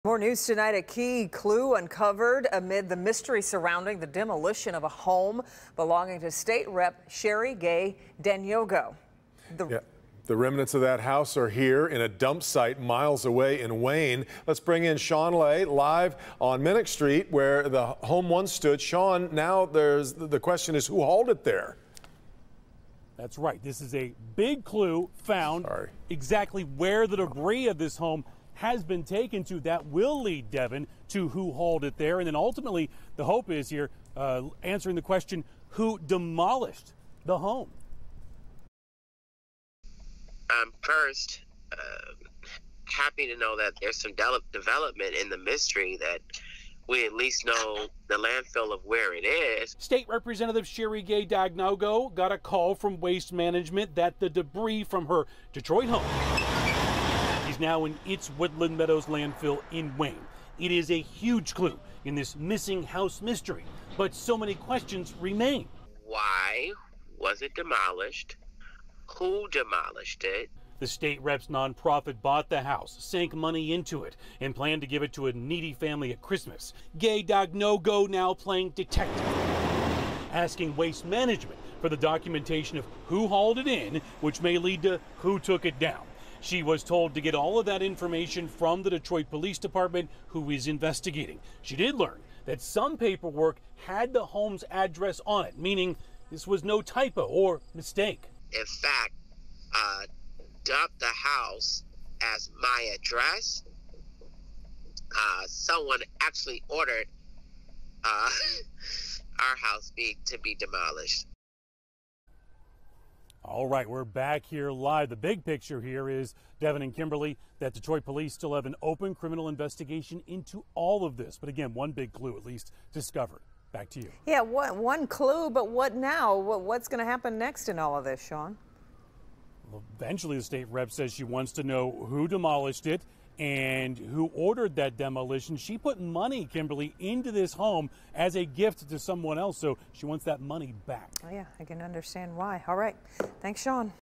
City of Detroit says it didn't order demolition of house meant for needy family
The city of Detroit said it didn't order the demolition of a home meant for a needy family.